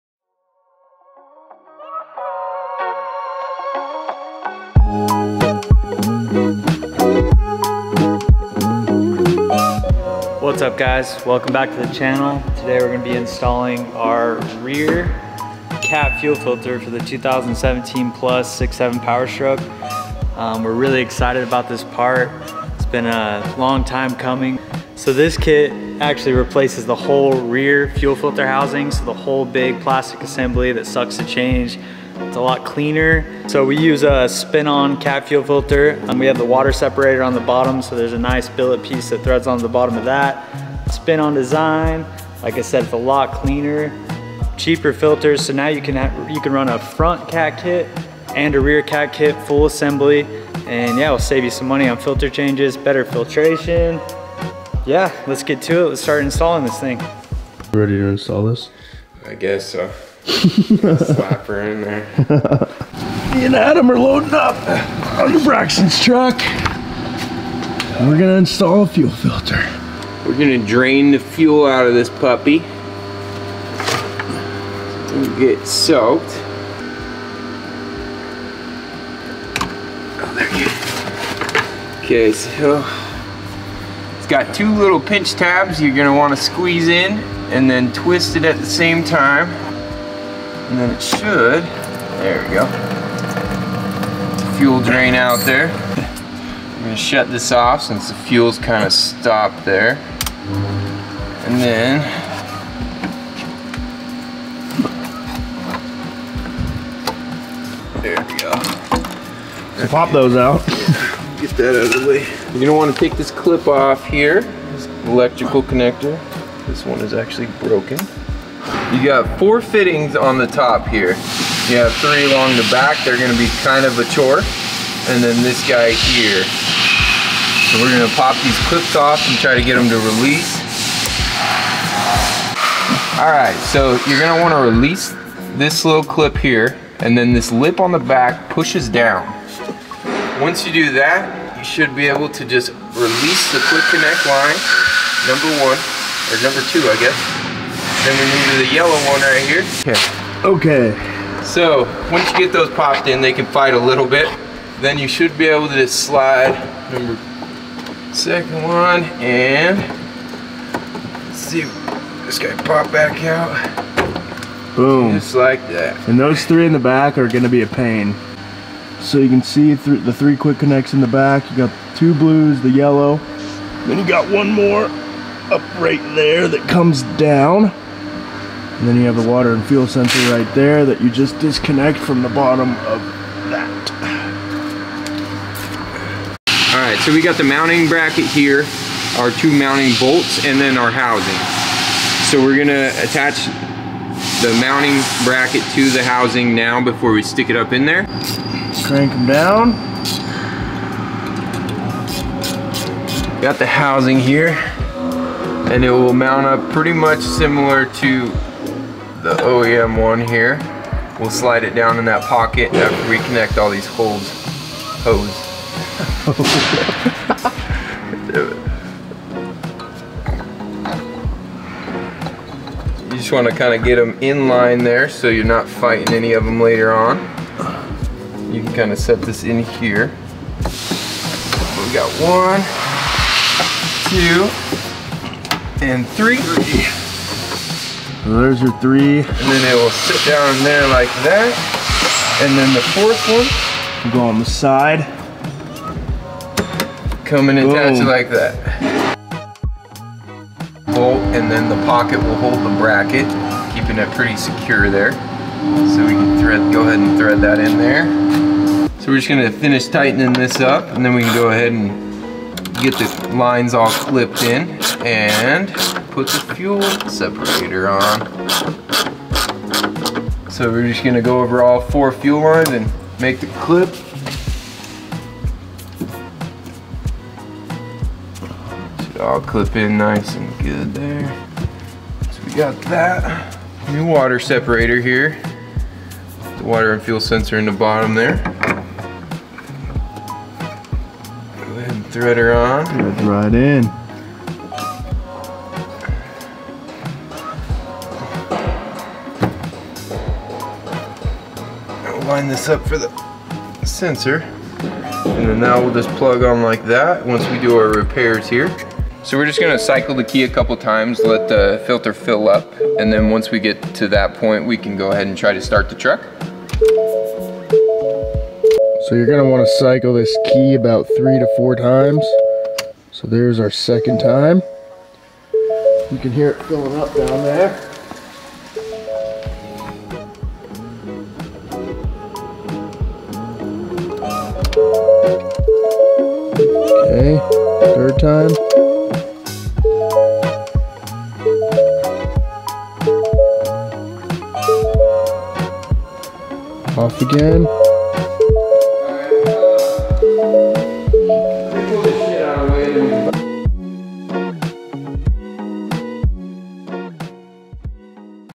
REAR CAT FUEL FILTER KIT ON 6.7 POWERSTROKE *INSTALL* BLACK MARKET PERFORMANCE
NEW BLACK MARKET PERFORMANCE PART!!

6.7 POWERSTROKE REAR CAT KIT!

EP.7

WE ARE BACK! TURN ON THOSE POST
NOTIFICATIONS!

DON'T MISS ANYTHING!